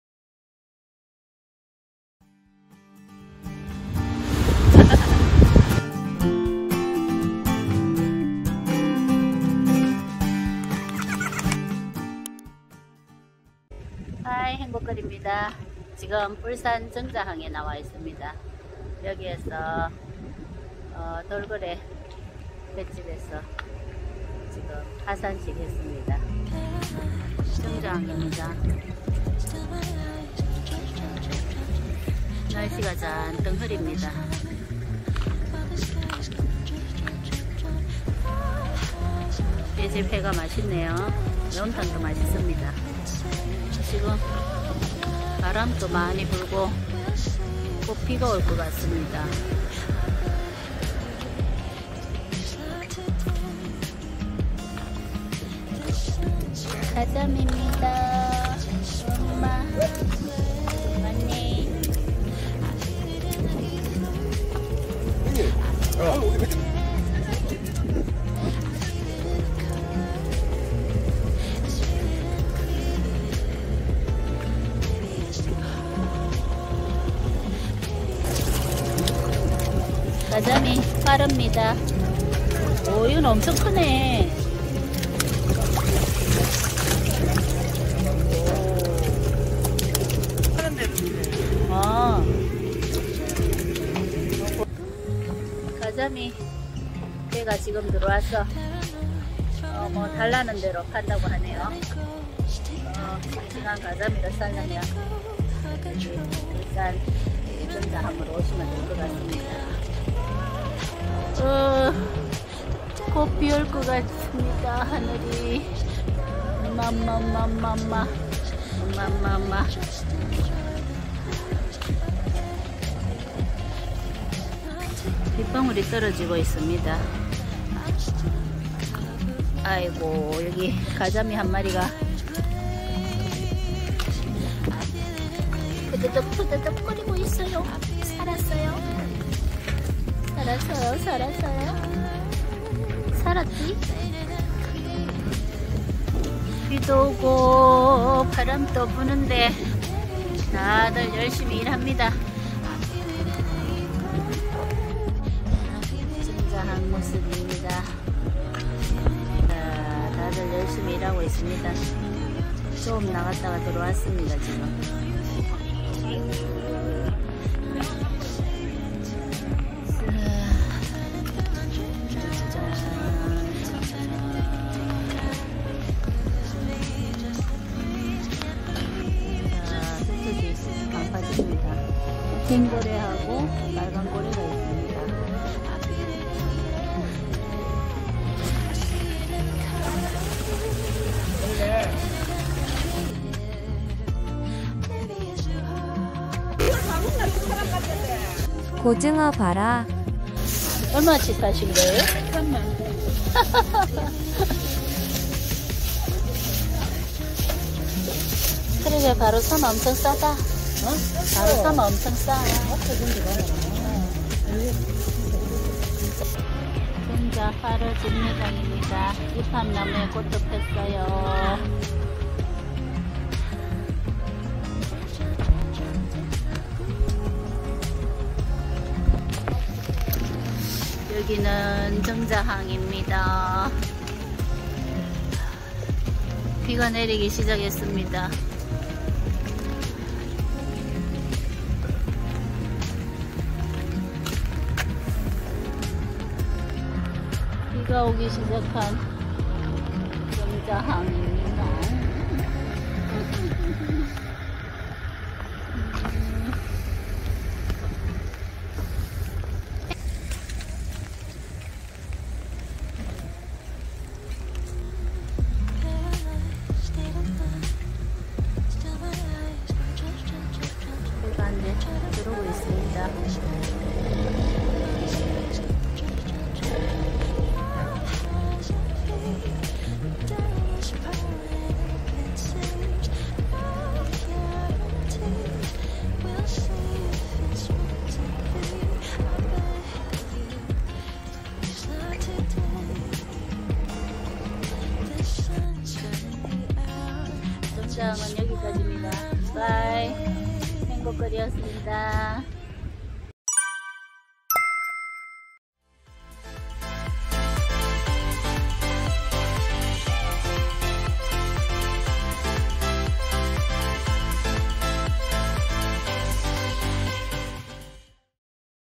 울산 북구 정자항에서 회와 매운탕 맛나게 먹기~~
오랜만에 정자항 가 봤습니다.
비가 오락가락하는데도 어민들은 열심히
생업에 종사하고 있었습니다.
요즘 가자미철인지 ?
씽씽한 가자미를 가득 잡아와서 사게 팔고 있어요.

무룡산 산행하고
식당에서 회와 매운탕 맛나게 잘 먹었습니다.